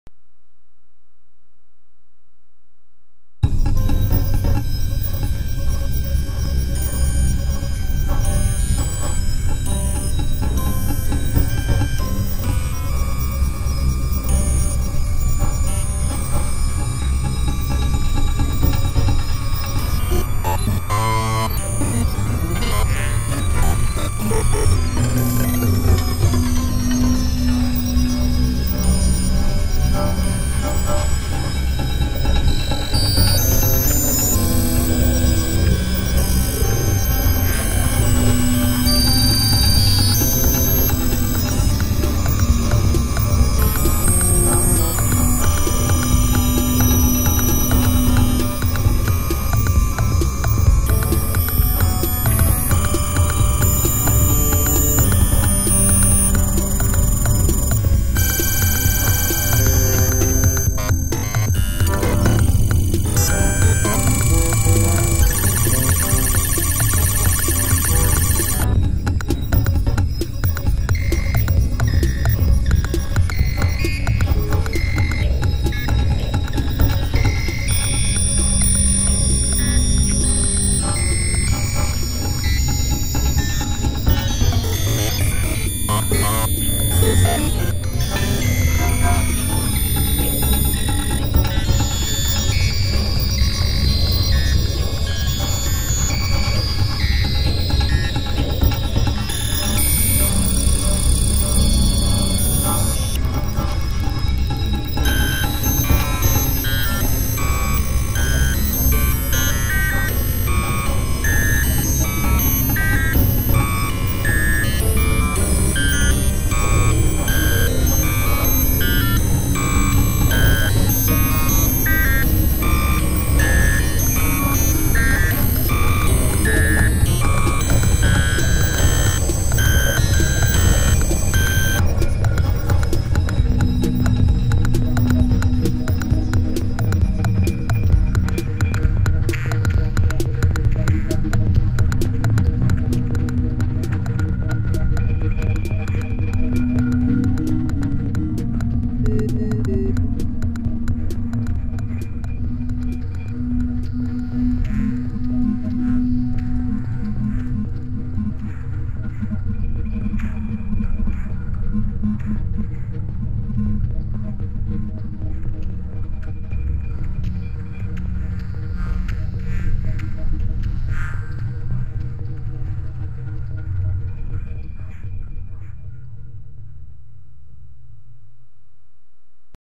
I am Nr4
The photos used for this visualisation are old stereo photos from the online archive of the US Library of Congress.
They show scenes from Istanbul, Hong Kong and Washington and have been shot around 1900.
The music is based on a rhythm pattern from an old TR-606, spiced up with sounds from a MIDI guitar. Arranged and mixed with Ableton Live.